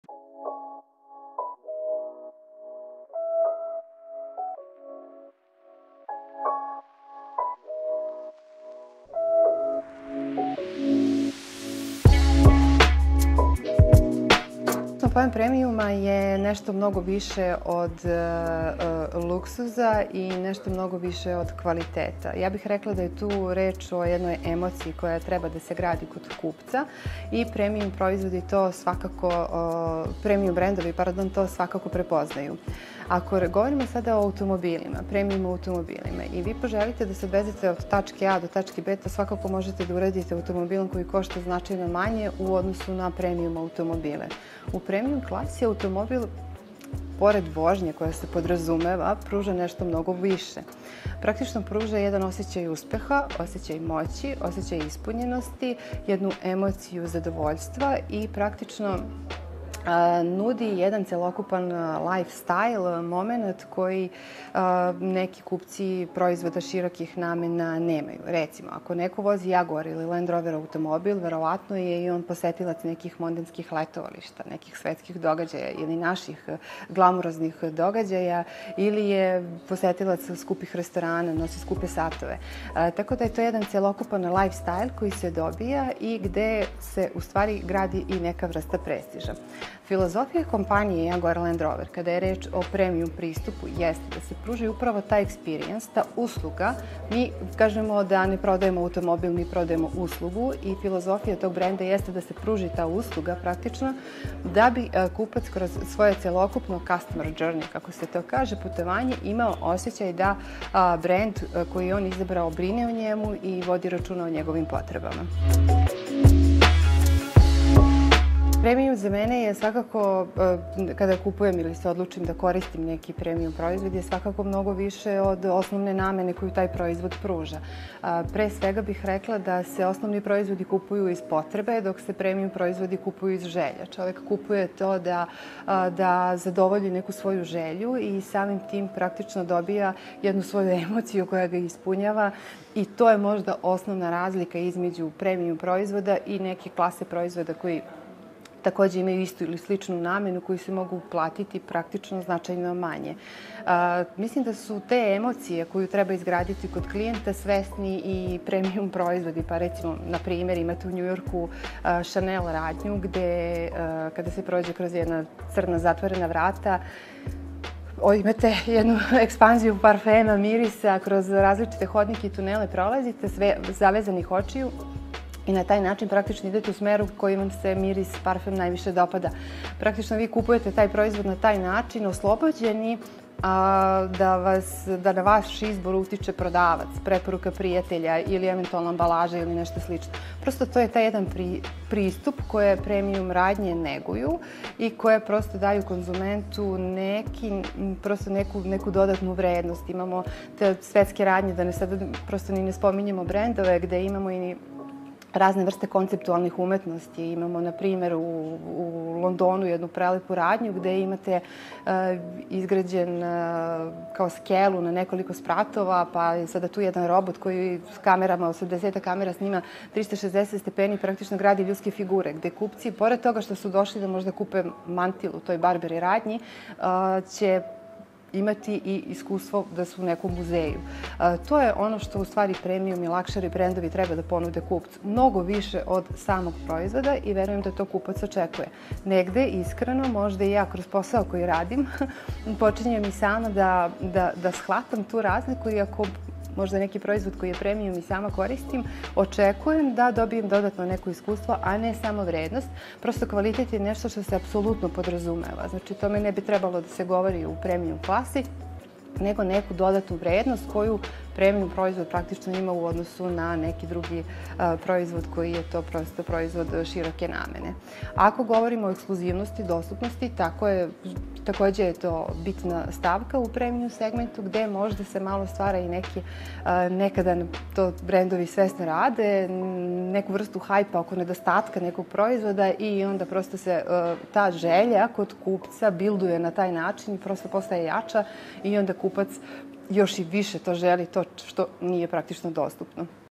BIZLife Premium večera powered by Miele: Vera Vuković
U intimnoj atmosferi livingrooma New Balkan Cuisine, BIZlife i Miele kompanija organizovali su prvu Premium večeru. Predstavnici premijum brendova otvorili su temu građenja i održavanja vlastitog imidža.

U ovom videu: Vera Vuković, menadžer marketinga kompanije British Motors

Premijum je izlazak iz stereotipa

Premijum je stvar klase, prestiža i emocija. Nešto što, pre svega, ukazuje na ekskluzivnost, dok se visok kvalitet apsolutno podrazumeva. Ovde nije reč o ceni, premijum proizvodi nisu proizvodi koji su neophodni, oni se kupuju iz želje, a ne iz potrebe.

Vi možete imati sasvim dobar automobil srednje klase, koji će vas prevesti od tačke A do tačke B, ali premijum automobil će vam pružiti mnogo više od vožnje. Reč je stilu, iskustvu, ekskluzivnosti, hedonizmu… Praktično, o jednom celokupnom životnom stilu, jer ako vozite automobil Jaguar Land Rover sigurno ste ljubitelj skupih satova, čest gost ekskluzivnih restorana, posetilac svetskih modnih revija ili mondenskih letovališta.